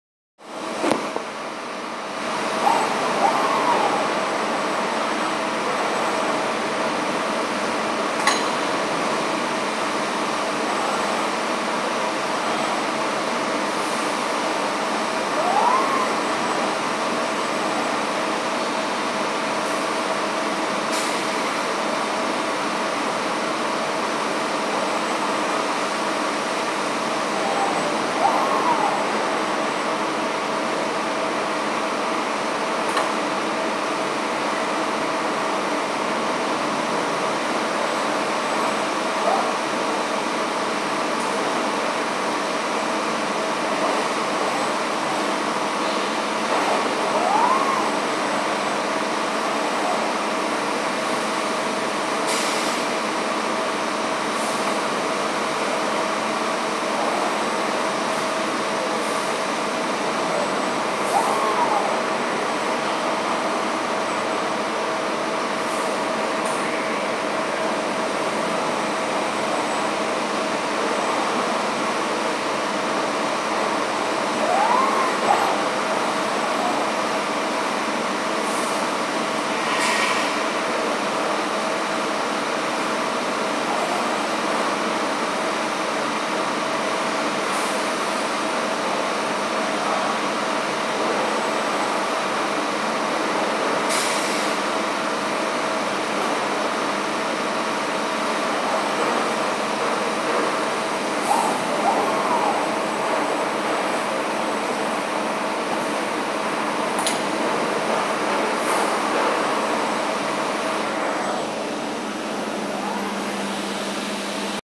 Mill Assist Essential With Turn Over Station
The Mill Assist has a static surface with grid plate allowing change between components in minutes.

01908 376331